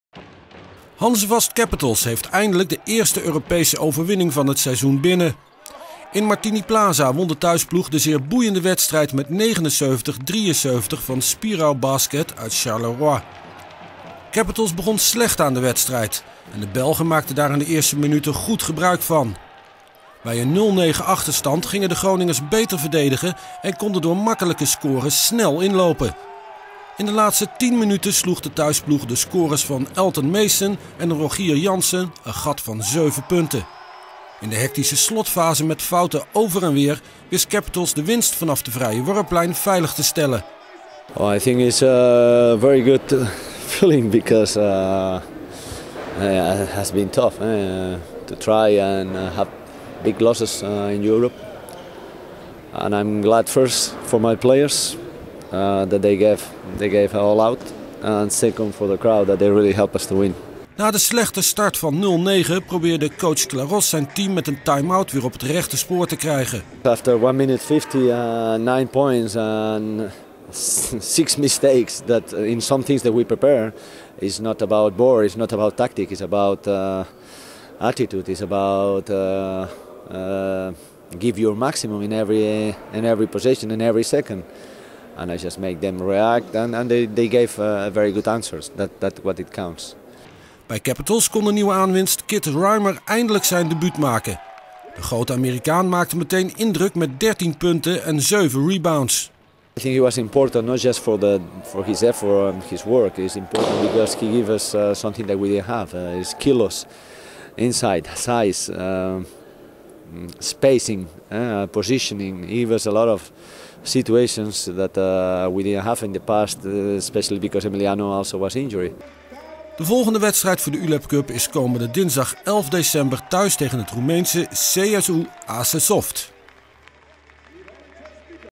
Capitals winnen eerste Europese wedstrijd
OOG TV Nieuws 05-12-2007 - Capitals winnen eerste Europese wedstrijd